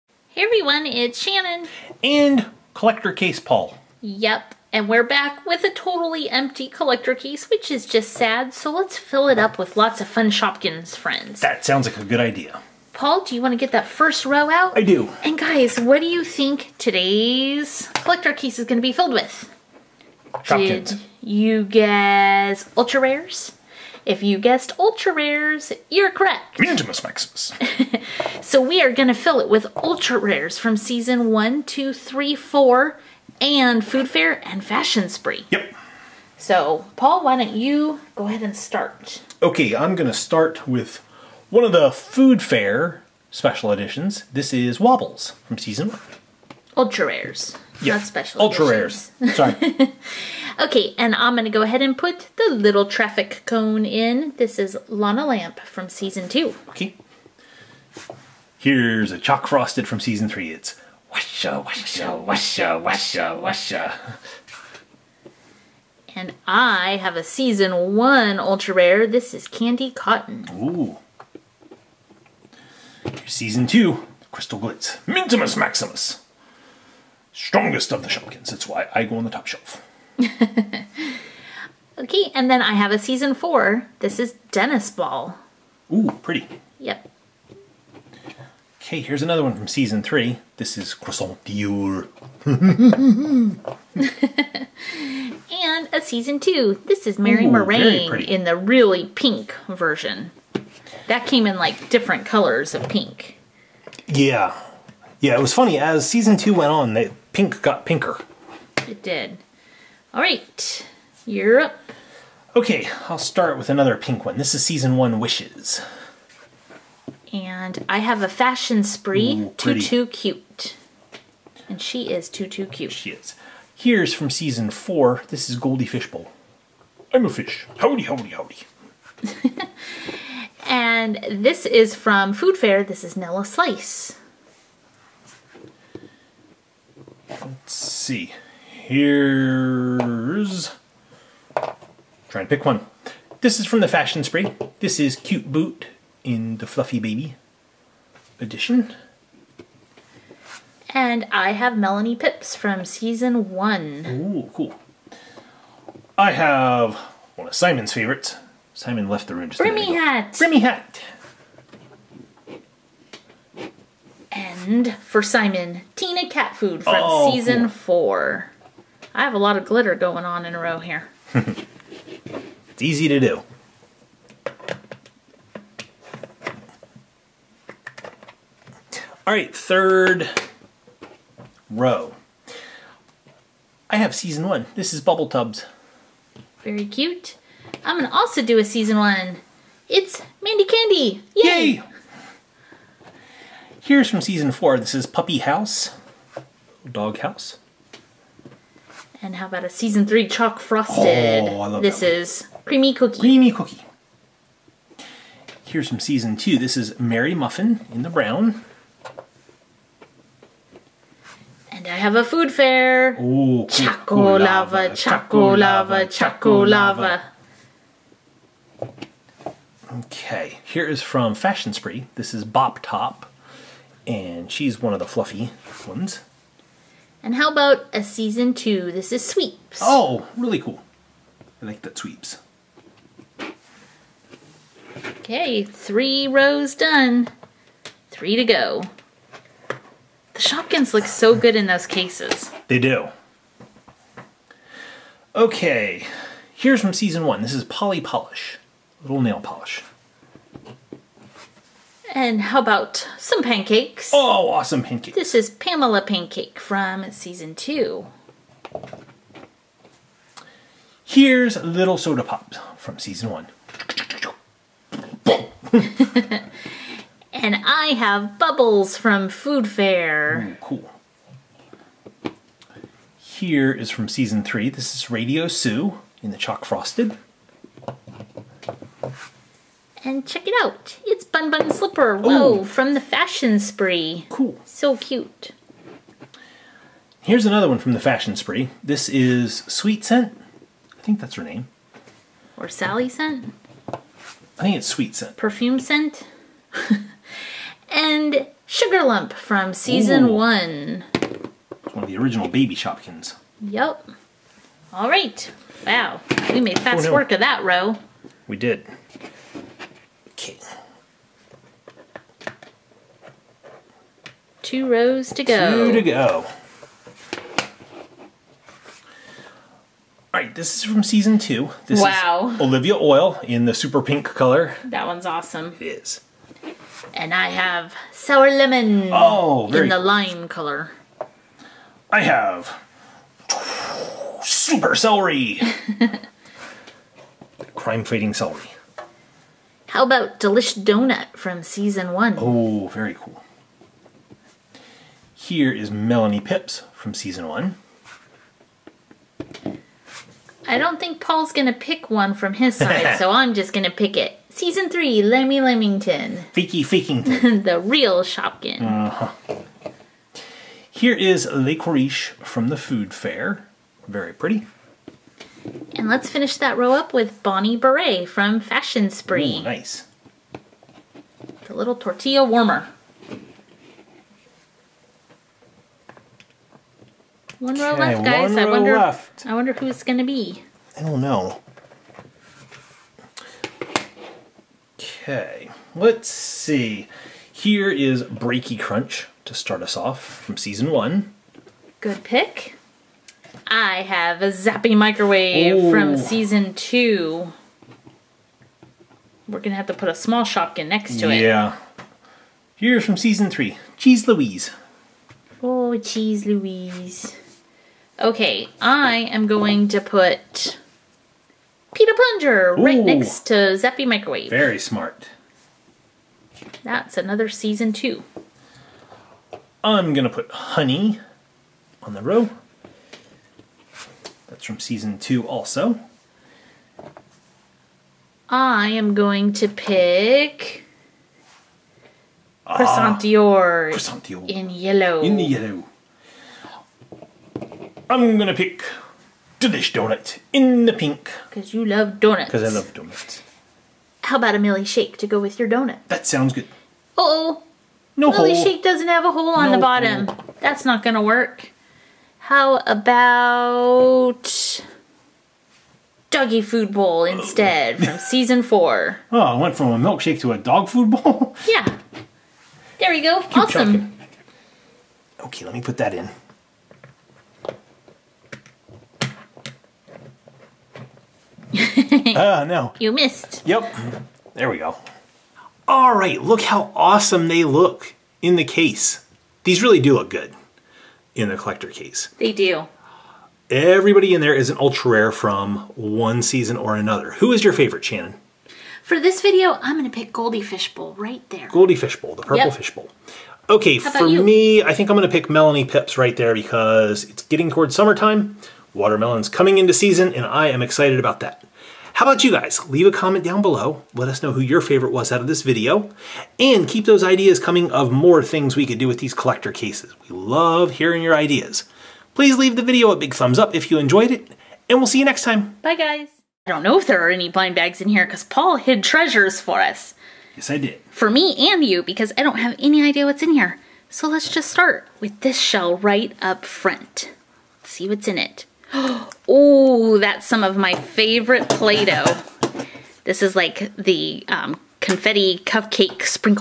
Shopkins Ultra Rare Collector Case Let's Decorate Season 1 2 3 4 | PSToyReviews
Today we have another suggestion from viewers, the Shopkins Collector Case ultra rare edition.  We are going to fill the case with only Ultra Rares from season 1, 2, 3 & 4.  Also Food Fair & Fashion Spree.  Let's see what Shopkins get picked for this case.